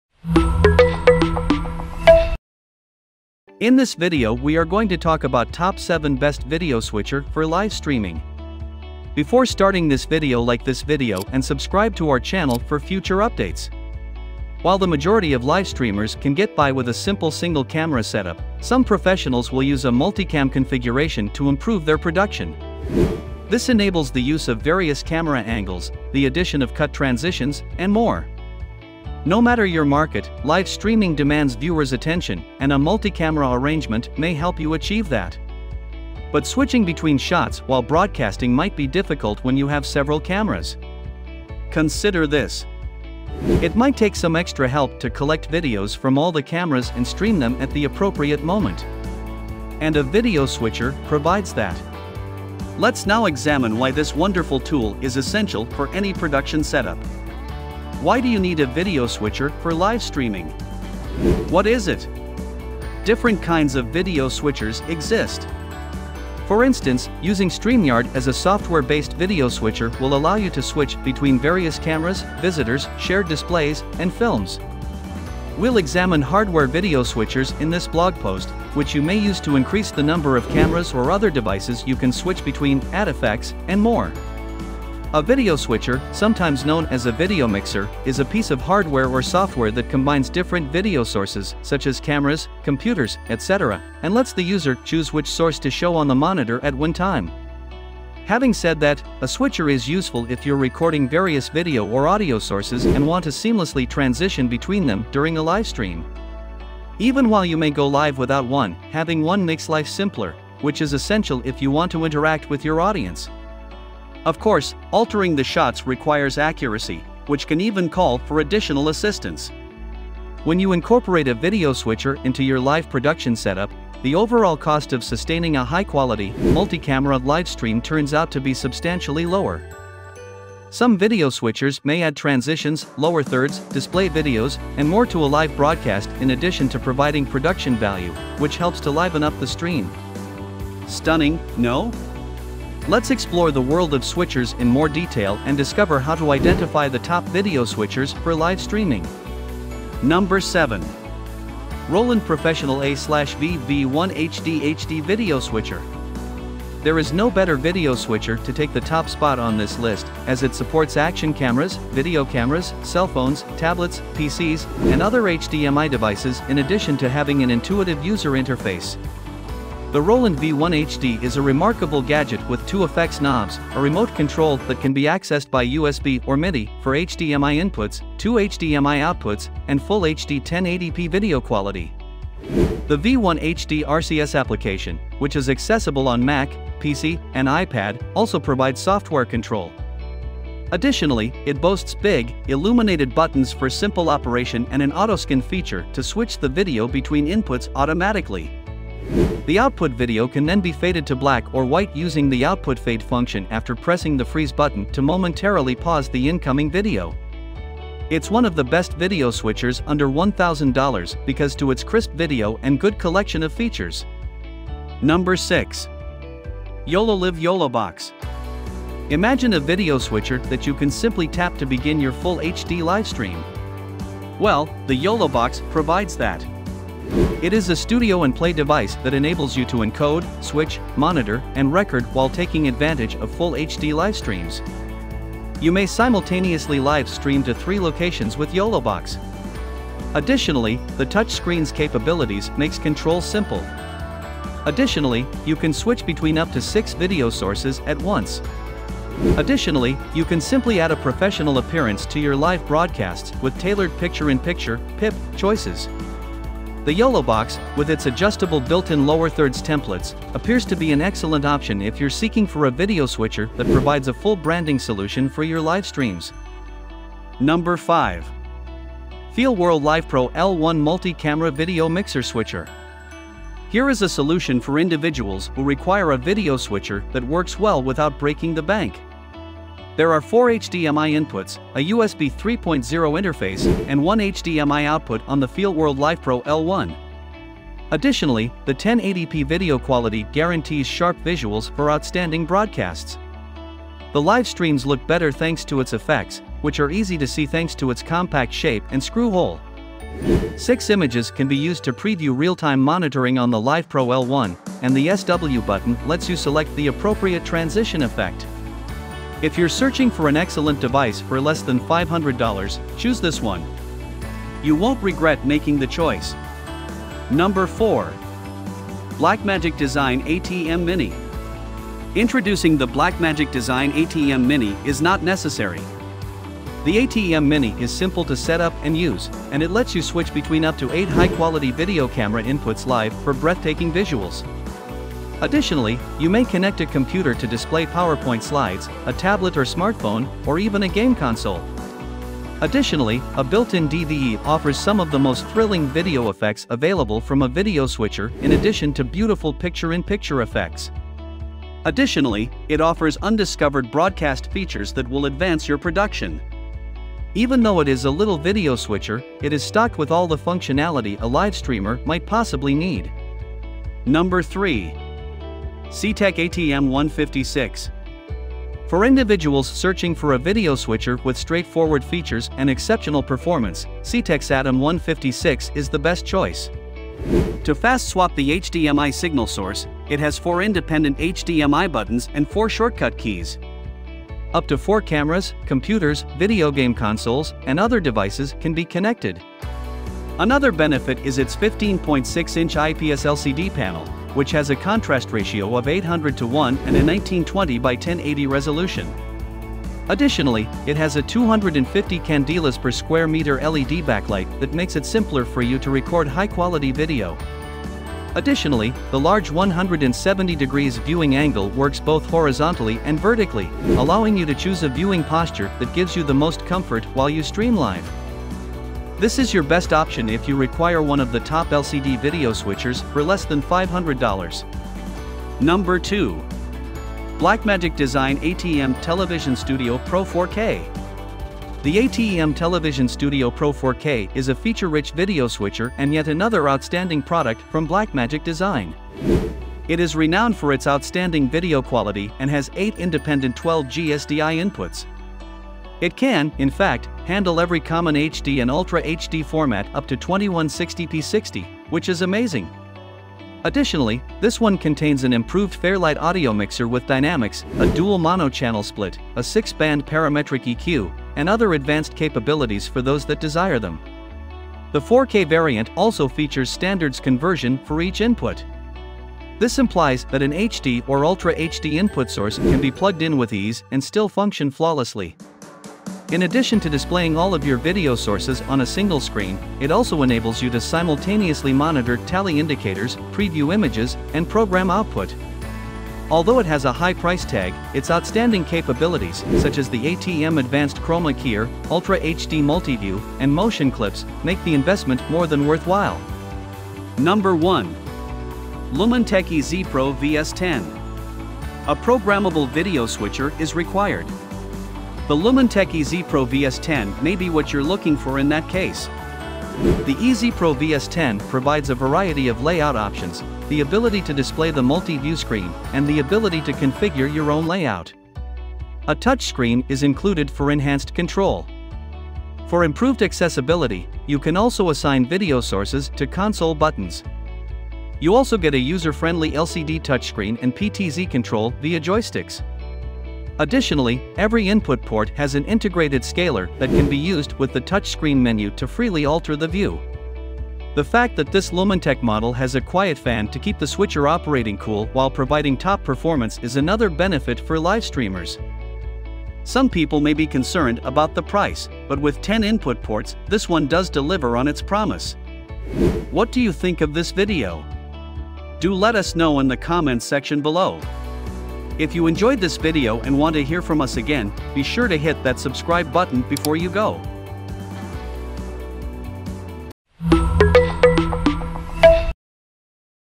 ✅ Top 7 Best Video Switchers for Live Streaming
⭐: 𝐖𝐄 𝐔𝐒𝐄 𝐀𝐌𝐀𝐙𝐎𝐍 𝐎𝐍𝐄𝐋𝐈𝐍𝐊, 𝐖𝐇𝐈𝐂𝐇 𝐖𝐎𝐑𝐊𝐒 𝐅𝐎𝐑 𝐌𝐎𝐑𝐄 𝐓𝐇𝐀𝐍 𝐅𝐈𝐕𝐄 𝐂𝐎𝐔𝐍𝐓𝐑𝐈𝐄𝐒.
Best Video Switchers for Live Streaming | Top 7 Best Video Switchers for Live Streaming
Product links: 👇👇👇

🔽7. 01:07 Roland Professional V-1HD
🔽6. 03:49 YOLOLIV YoloBox
🔽5. 04:21 FEELWORLD LIVEPRO L1
🔽4. 05:07 Blackmagic Design ATEM Mini Pro
🔽3. 06:06 Seetec Atem156
🔽2. 07:15 Blackmagic Design ATEM Television Studio Pro 4K
🔽1. 08:35 Lumantek ez-Pro VS10

                        ˜”*°•.˜”*°• 𝐋𝐢𝐯𝐢𝐧𝐠 𝐒𝐩𝐞𝐚𝐤𝐞𝐫 •°*”˜.•°*”˜